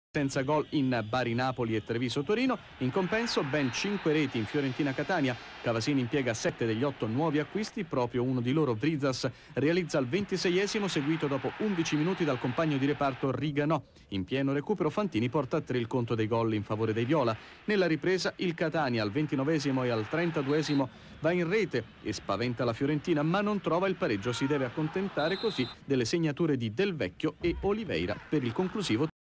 Fiorentina-Catania 3-2 Serie B 03-04 21' Giornata
26'  VRYZAS                 65'  DELVECCHIO
41'  RIGANO'                 84'  OLIVEIRA
46'  FANTINI